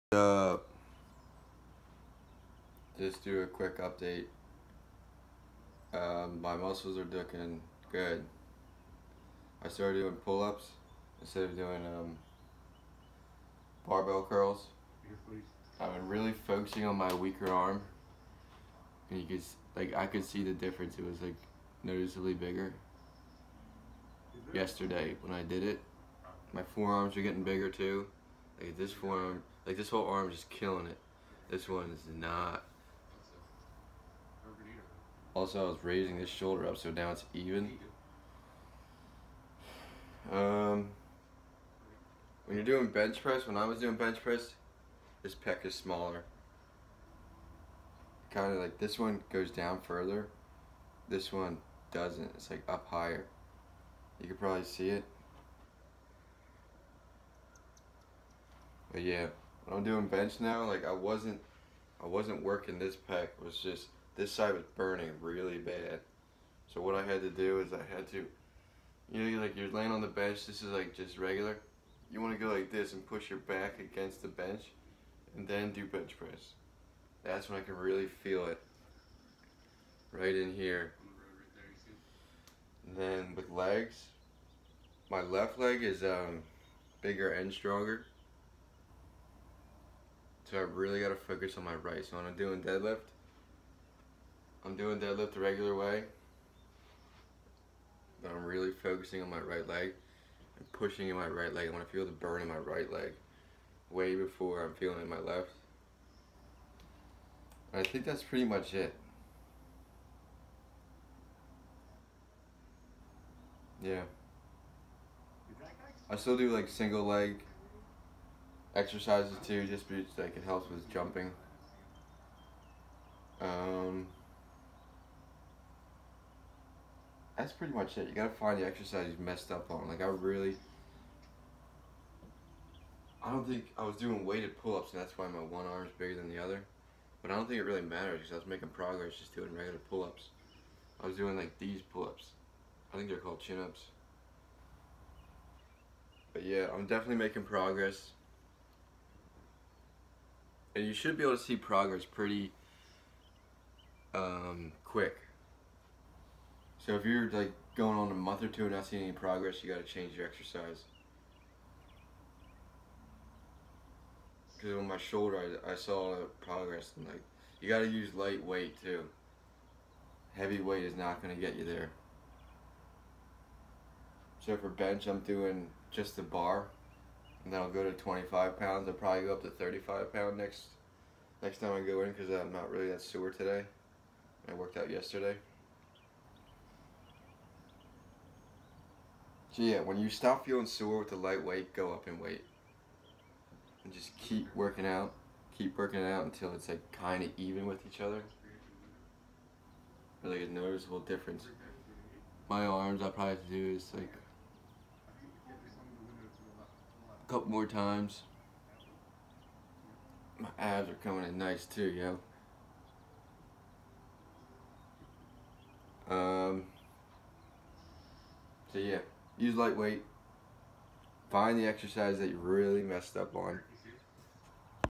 Muscles bigger on one side UPDATE 6/23/18
Muscles bigger on one side update 6/23/18. I take you guys on a wild ride.

Here we talk about chest, biceps, and legs.

Your muscles are bigger on one side due to poor lifting when you were younger